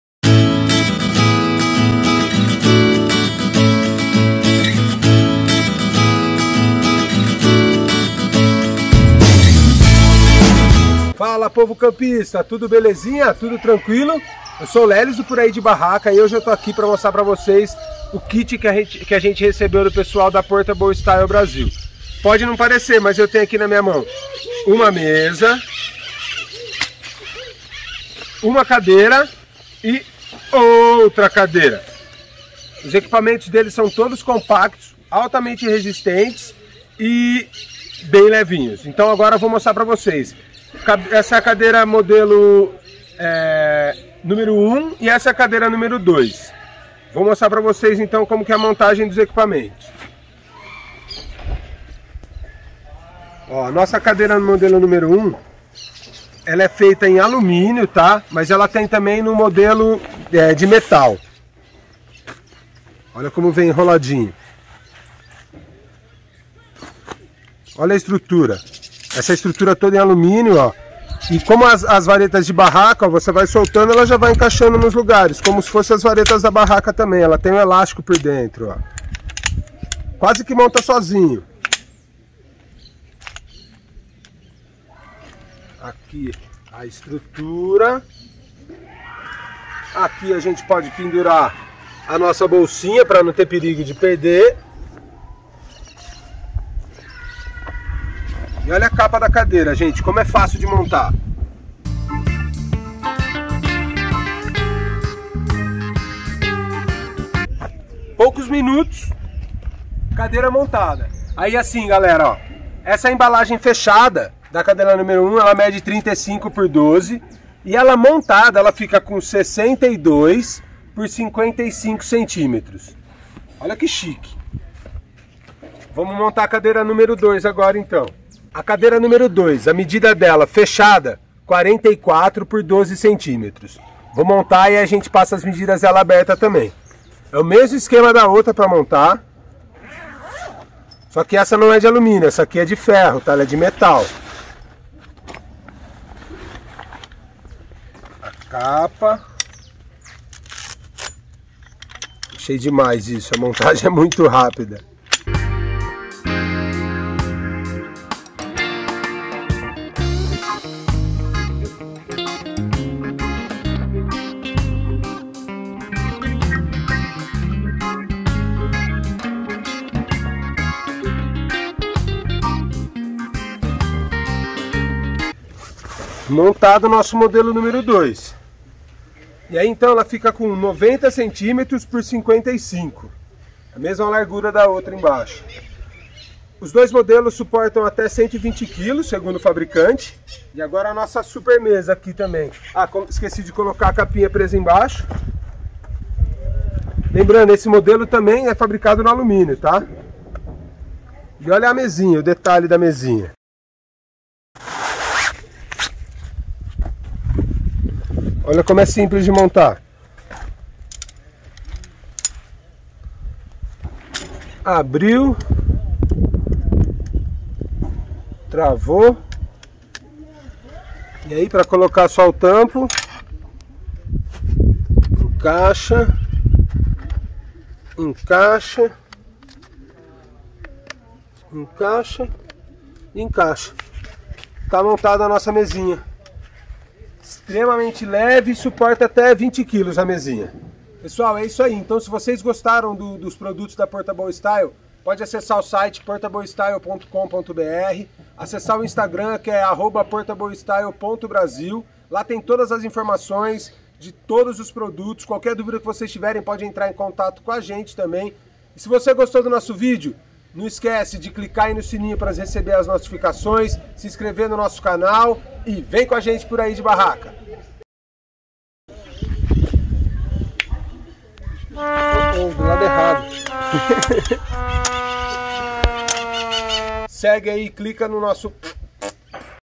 Kit Portable Style
Recebemos este kit de cadeiras e mesa da Potable Style e ficamos impressionados com sua montagem e peso dos materiais.
A cadeira de fácil e rápida montagem, indicada para grandes aventuras que necessitam de praticidade e economia de peso.
A mesa é compacta e de fácil montagem, este modelo torna-se ideal para quem procura uma mesa prática e que seja leve em suas aventuras.
• Por aí de barraca na internet •